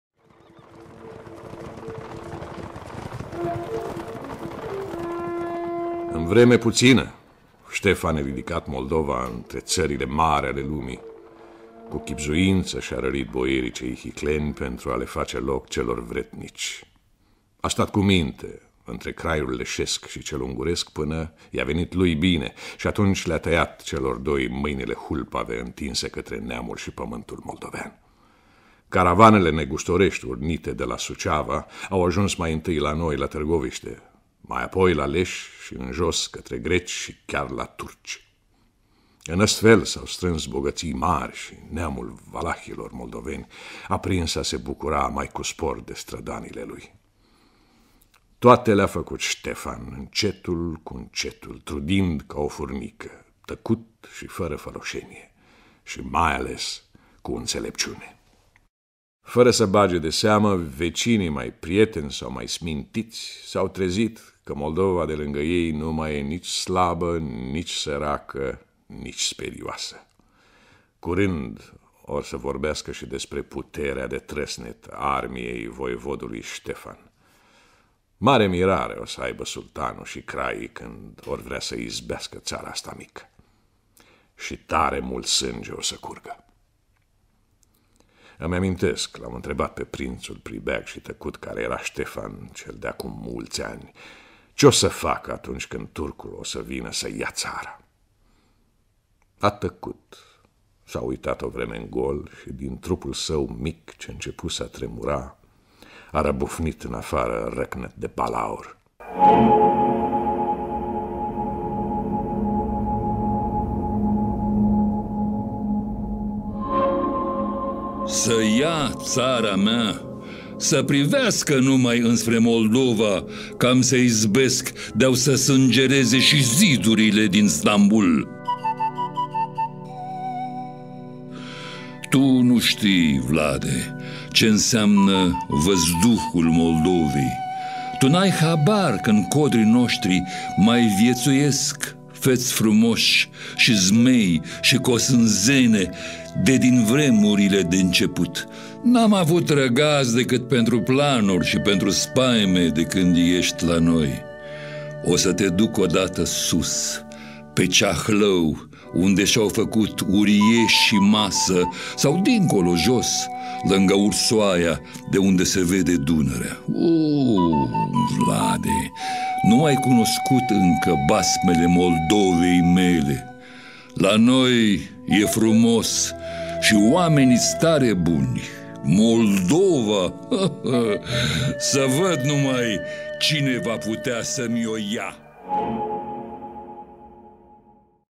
Furia Zimbrului
Trei titani ai culturii romane isi unesc puterile magice  in cartea audio Furia Zimbrului de Vasile Lupasc.  Interpreteaza Mircea Albulescu si Marcel Iures. Muzica Gheorghe Zamfir.